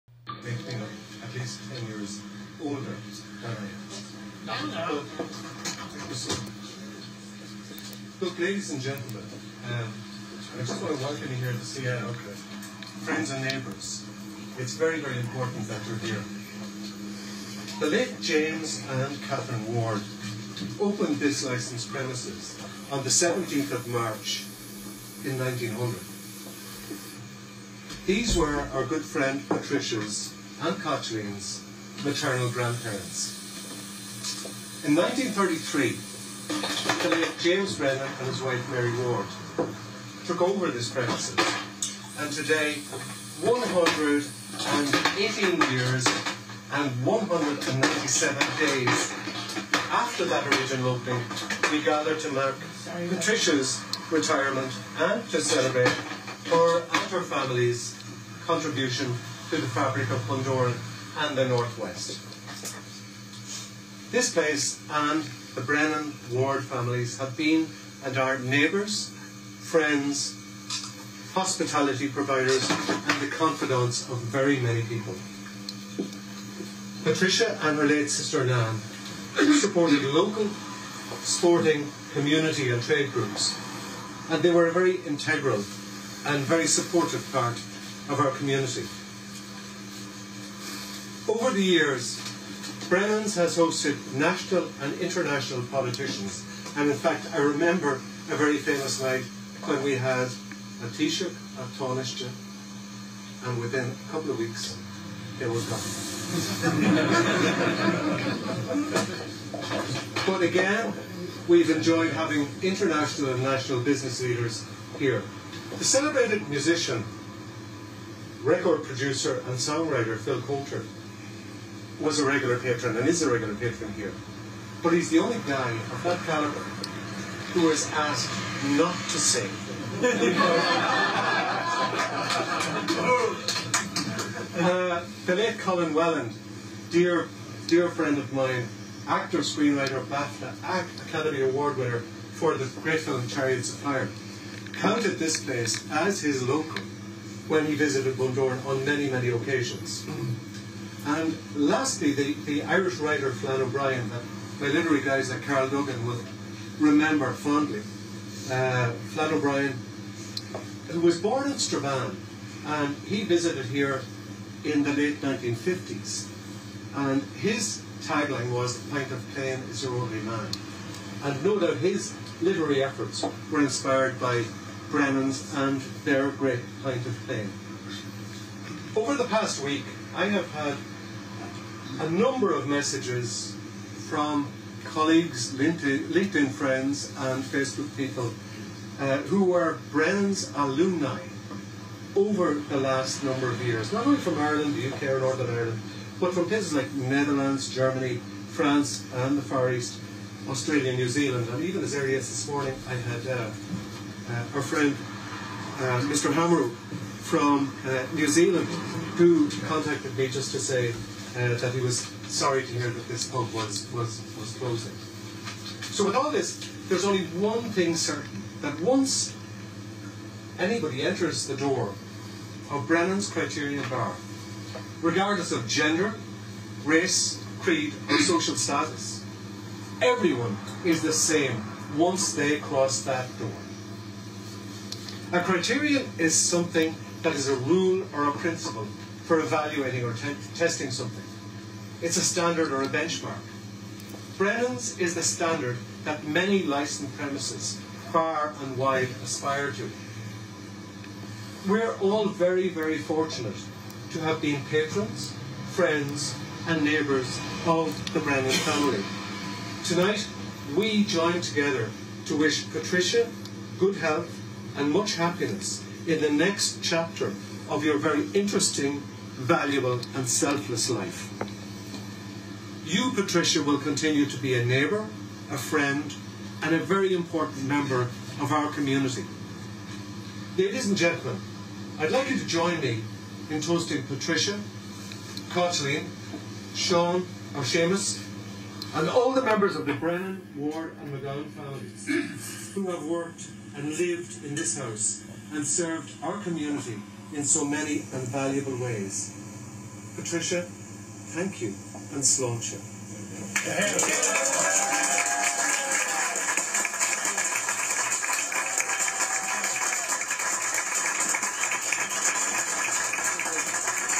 Cormac Meehan Brennan';s Criterion bar of Bundoran closes
Patricia Brennan';s Criterion bar of Bundoran closed end of September 2018, speech by Cormac Meehan